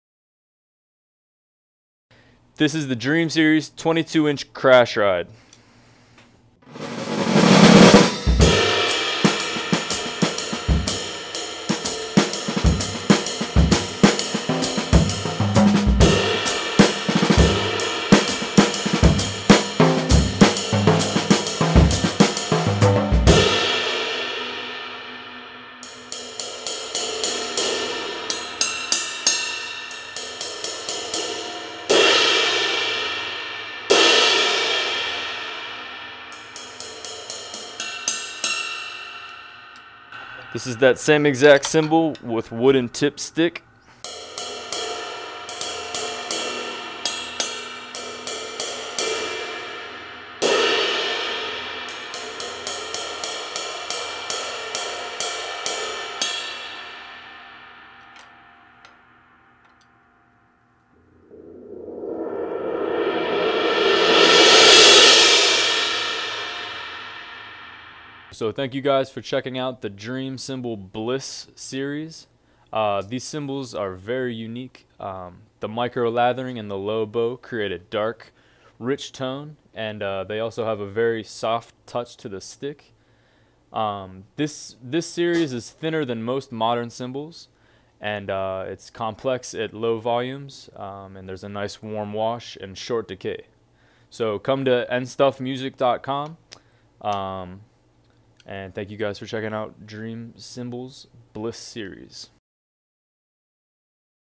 Dream Cymbals Bliss 22" Crash/Ride - BCRRI22
Dream Cymbals BCRRI22. Bliss series are new cymbals made in the ancient tradition. Every one is an individual work of art, painstakingly hand forged, and hand hammered.

They have a warmth that seduces at low volume and thunders with a froth of dark undertones at more extreme volumes. These cymbals come alive, shimmering and wobbling as if the mere touch of human hands excites the cymbal and it can't wait to get it's voice out there and sing.

They are exceptionally resilient and slightly soft on the stick. The undertones are warm, rich, dark with an edge of dragon's breath. Each one is a unique, complex voice that will excite your imagination.

These cymbals are so versatile. Quite thin, they produce explosive crashes that get out of the way with medium decay, a soft but clear stick sound, clear bell tone and that coveted edge wobble. .

468 Freeport Road
Pittsburgh PA 15238